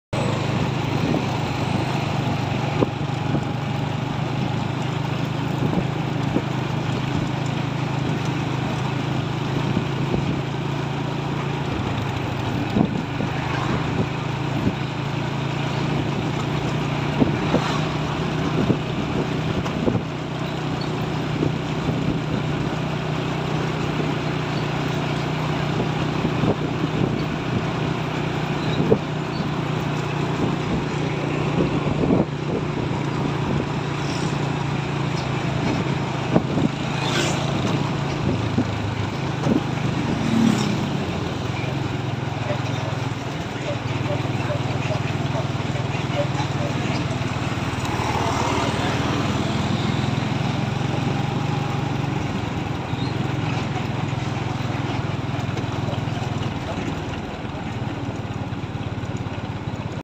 YouTube Shorts truck oleng membawa jeruk bali#jelas production#shorts
Truk ini kendaraan yang sedang kirim buah jeruk bali ke salah satu pasar dengan lampu yang indah, seperti ini pasti disukai para penggemar truckmania#shorts